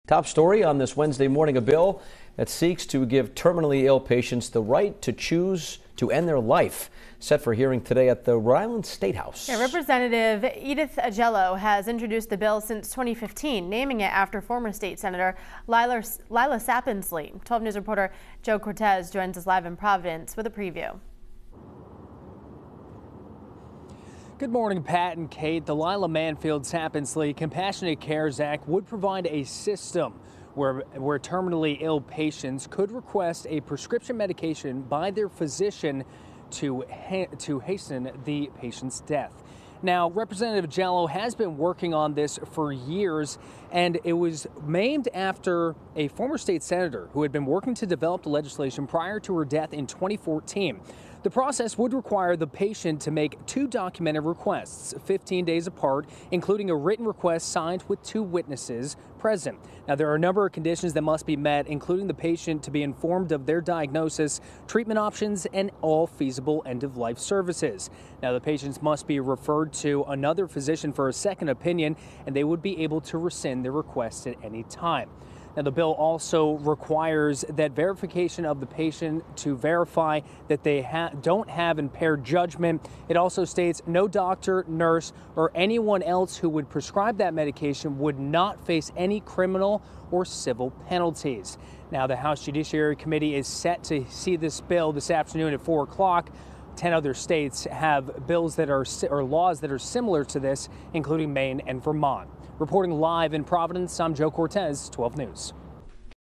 RI lawmakers to hear assisted suicide bill for terminally ill patients
Rhode Island lawmakers are set to hear a bill on Tuesday that looks to give terminally ill patients the right to choose to end their life.